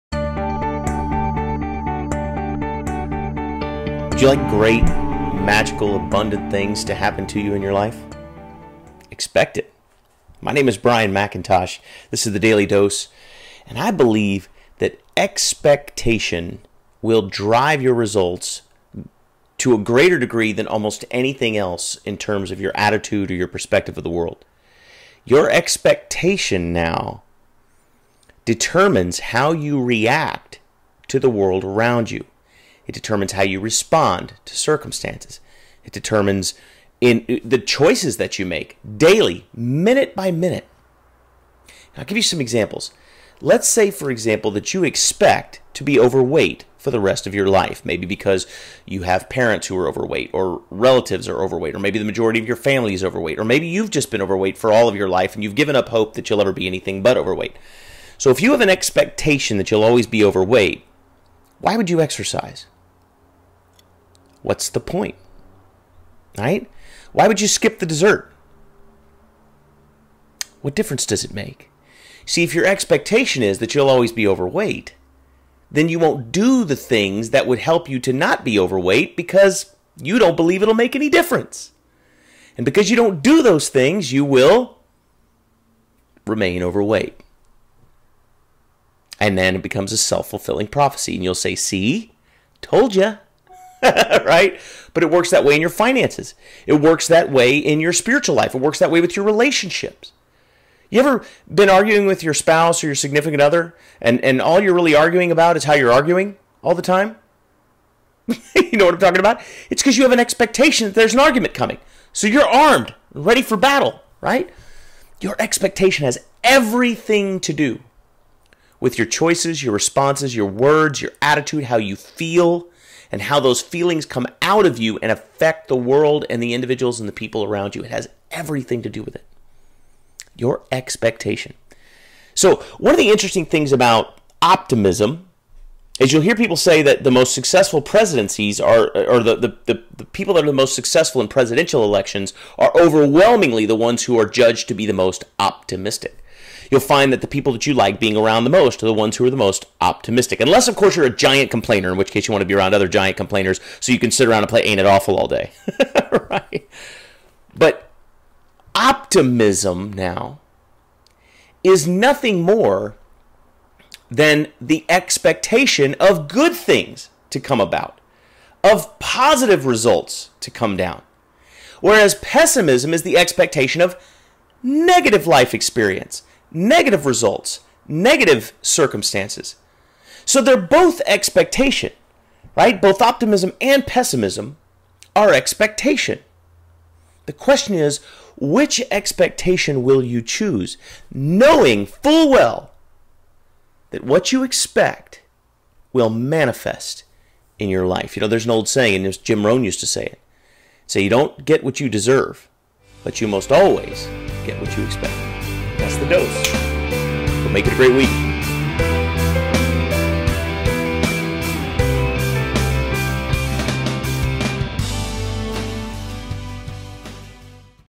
Expectation: the science behind OPTIMISM
Expectation is the science behind optimism.  What you expect will drive your emotions, your actions, your decisions, and ultimately your circumstances.  Think I'm over exaggerating the effects of expectation?  Listen to this: